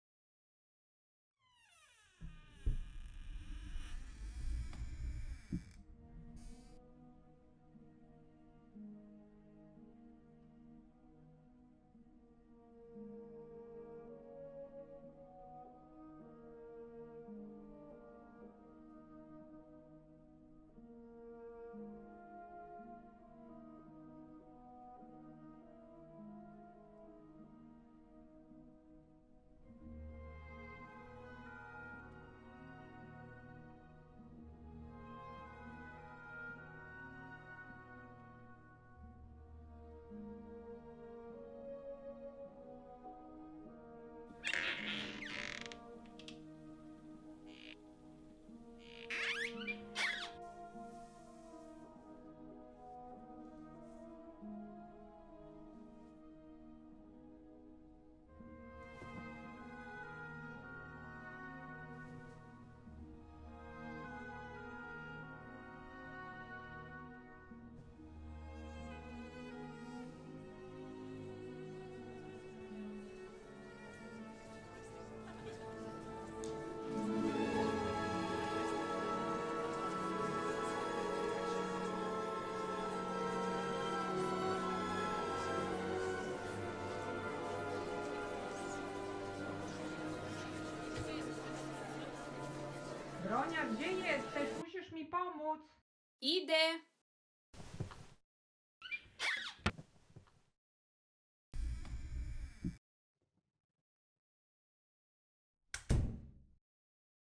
Scene inspired by "The Chosen Maiden" - Eva Stachniak
Scene inspired by Eva Stachniak's book, "The Chosen Maiden"
Written and directed by Alessia D'Angelo
Camera operator and DOP: Davide Papadia
Cast: Alessia D'Angelo, Lisa Colombo, Edyta Cackowska
Sound: Federico Erba